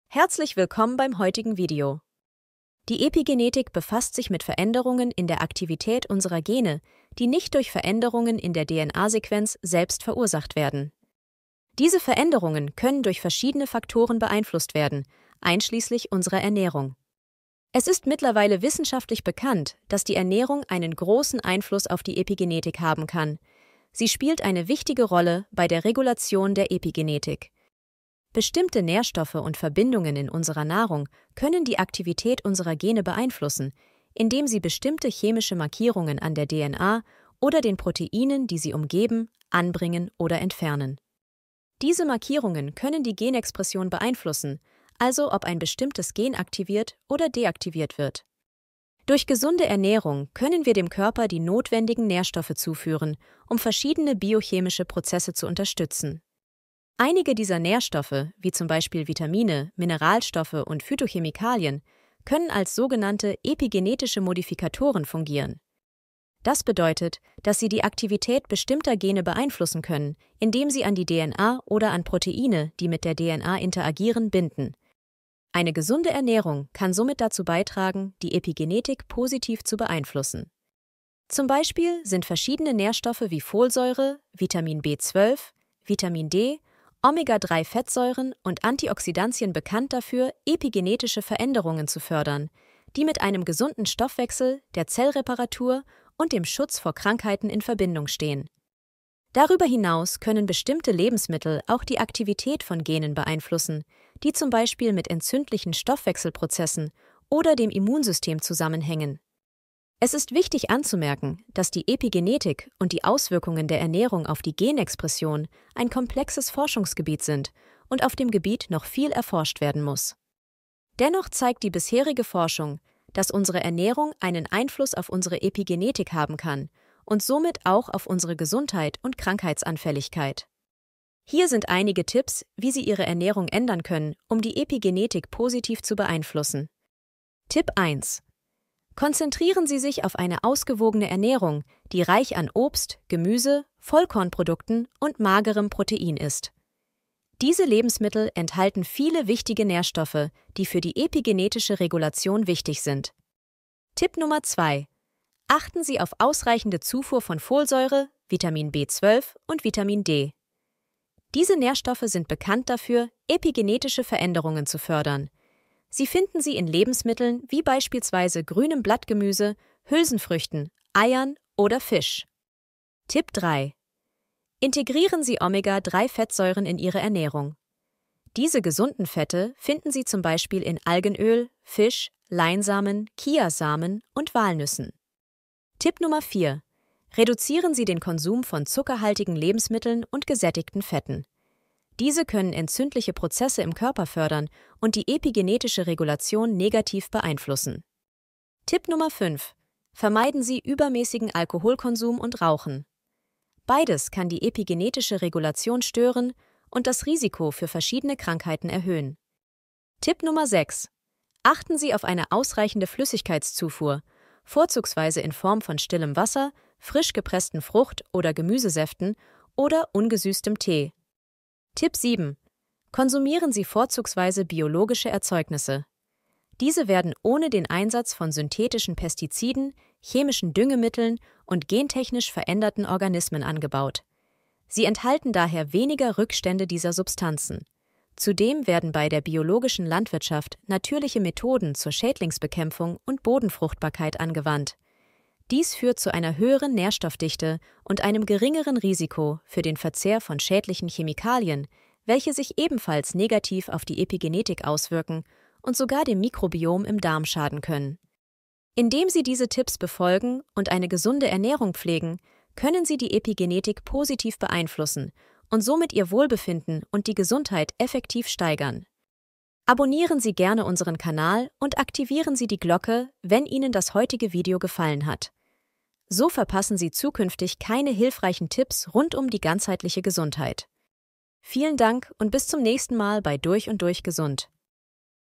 Die Macht der Ernährung - Wie wir unsere Gene durch Epigenetik steuern können
Im heutigen Video tauchen wir ab in die Welt der Epigenetik und erklären grundsätzliches, wie unsere Ernährung einen direkten Einfluss auf unsere Gene haben kann.

Haben Sie sich schon einmal gefragt, warum manche Menschen bestimmte Krankheiten entwickeln, während andere gesund bleiben? Die Antwort könnte in der Epigenetik liegen - einem spannenden Forschungsbereich, der sich mit den Veränderungen der Genaktivität befasst, die nicht auf Veränderungen der DNA-Sequenz zurückzuführen ist. Und die gute Nachricht ist: Wir können diese Veränderungen durch unsere Ernährung beeinflussen!

In diesem Video werden wir die Grundlagen der Epigenetik erläutern und herausfinden, wie bestimmte Nährstoffe und Lebensmittel unsere Gene aktivieren oder deaktivieren können. Desweiteren werden Sie erfahren, wie ungesunde Ernährungsgewohnheiten das Risiko für Erkrankungen und Stoffwechselstörungen beeinflussen können.

Darüber hinaus geben wir einige praktische Tipps und Ratschläge, wie Sie ihre Ernährung anpassen können, um ihre Gene positiv zu beeinflussen und somit ihre Gesundheit nachhaltig verbessern können. Von der Bedeutung einer ausgewogenen Ernährung bis hin zu spezifischen Nahrungsmitteln und Nährstoffen, die epigenetische Veränderungen positiv fördern, erklären wir die Grundlagen.

Begleiten Sie uns auf dieser aufschlussreichen Reise durch die Welt der Epigenetik und entdecken Sie die erstaunliche Macht, die wir haben, unsere Gene durch unsere Ernährung zu steuern. Verpassen Sie nicht die Chance, ihr Wissen zu erweitern und ihren Lebensstil zu optimieren, um ein gesünderes und erfüllteres Leben zu verwirklichen.

Sofern diese mit Quellenangabe zu diesem Video versehen sind.